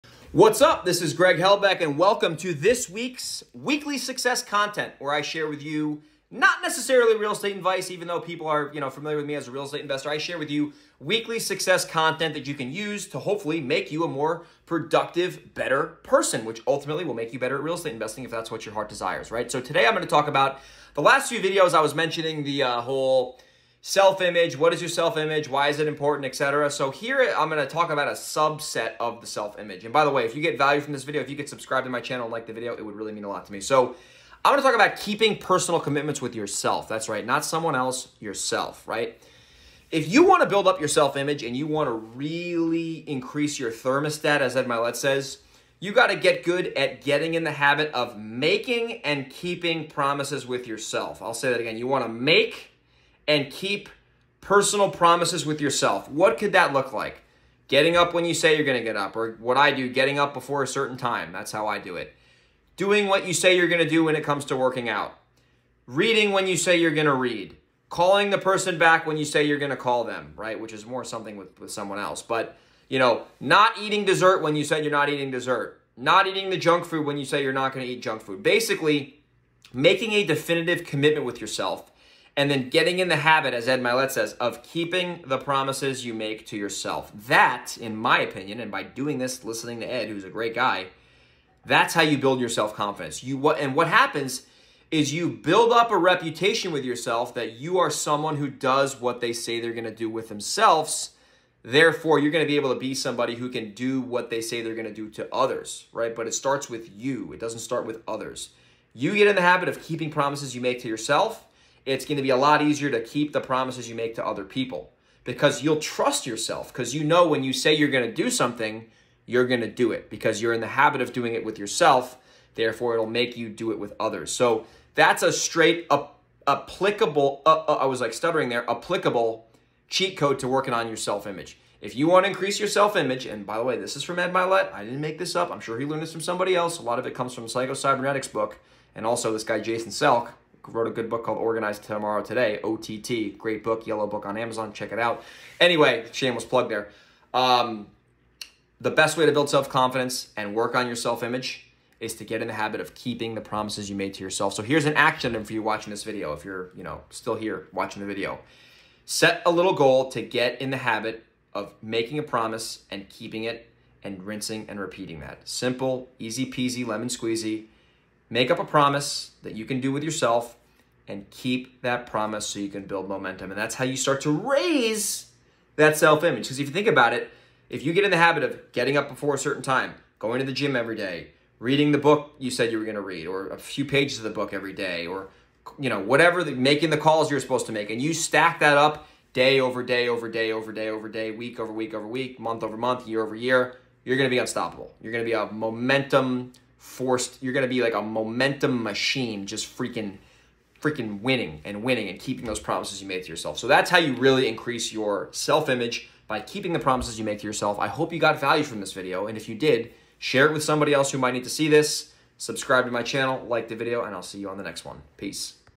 Keeping Personal Commitments With Yourself | Weekly Success Tip
In this week's Weekly Success Tip video, I talk about how to keep personal commitments, and how doing so can build your confidence and create more positive interactions with others.